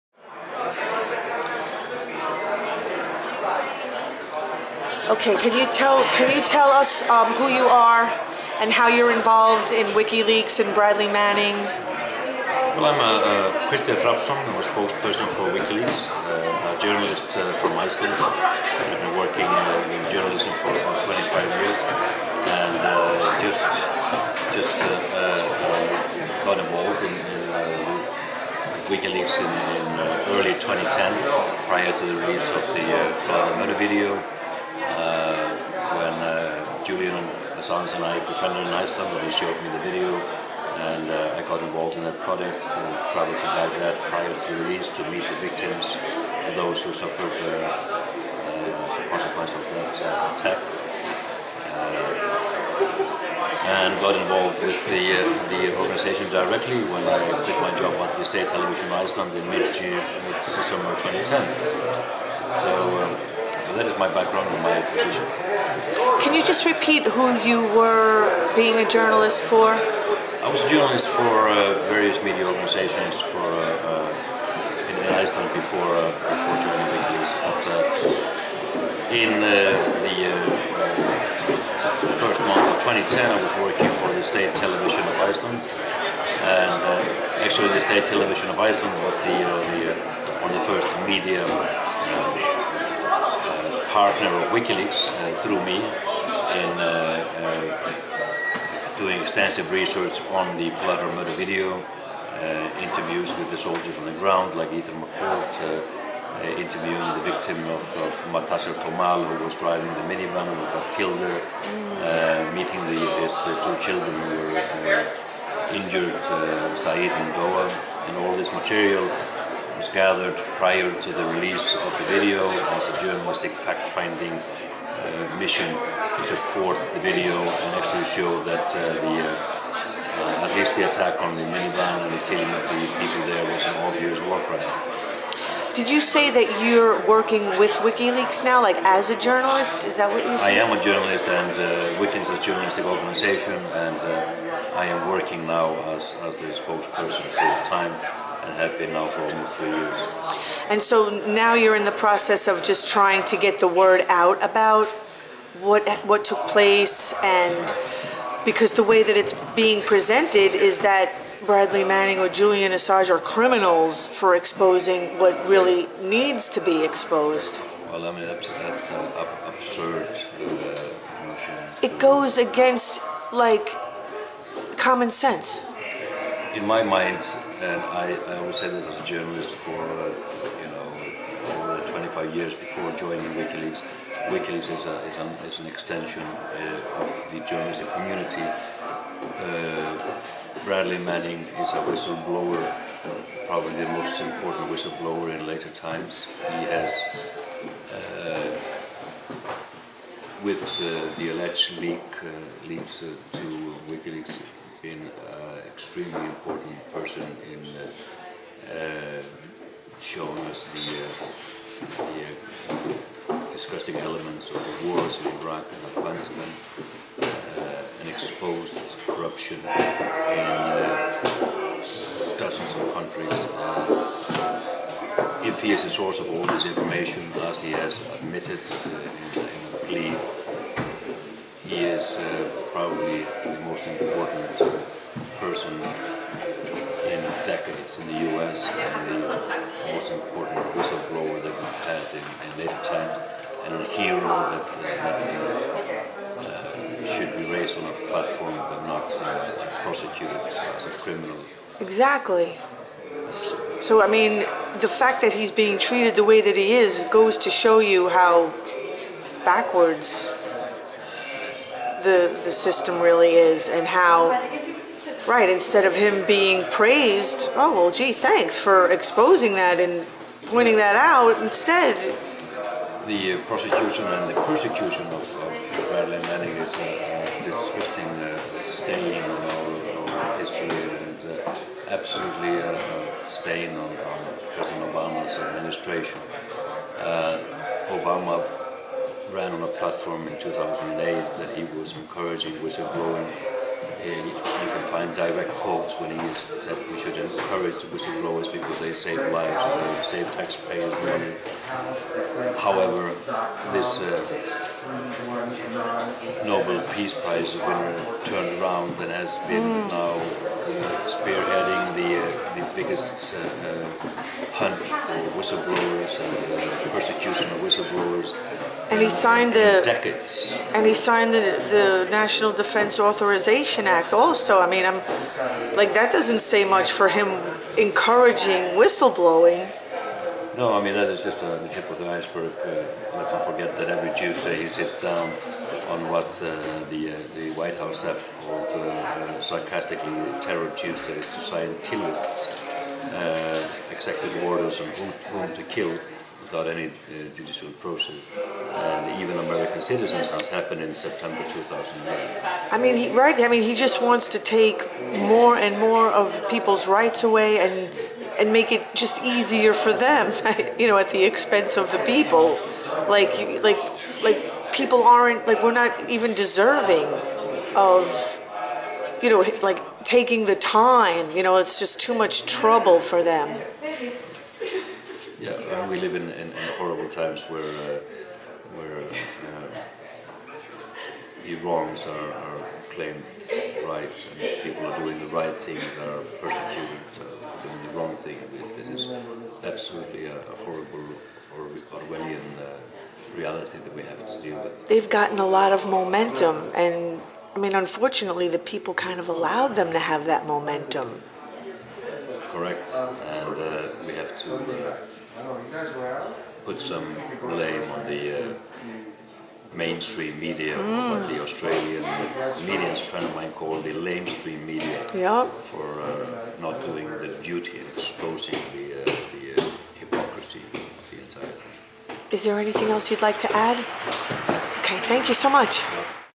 Interview/ Icelandic Wikileaks Journalist, KRISTINN HRAFNSSON
Interview with Icelandic Journalist Kristinn Hrafnsson who I met at a Great Bradly Manning event in NYC. Thanks to Greg Palast!

Krisstin is an Icelandic investigative journalist and spokesperson for the WikiLeaks organisation.

He has worked at various newspapers in Iceland and hosted the television programme Kompás on the Icelandic channel Stöð 2, where he and his team often exposed criminal activity and corruption in high places. In February 2009, while investigating the connection between Iceland's Kaupthing Bank and Robert Tchenguiz, the programme was taken off air and Kristinn and his crew were sacked.

Shortly thereafter, Kristinn was hired by RÚV, the Icelandic National Broadcasting Service. In August 2009, he was working on a story about Kaupthing's loan book which had just been published on the WikiLeaks webpage, when the bank got a gag order issued by the Reykjavik sheriff's office, banning RÚV from reporting on the loan book, which could be publicly accessed online via WikiLeaks. The prohibition order was withdrawn later.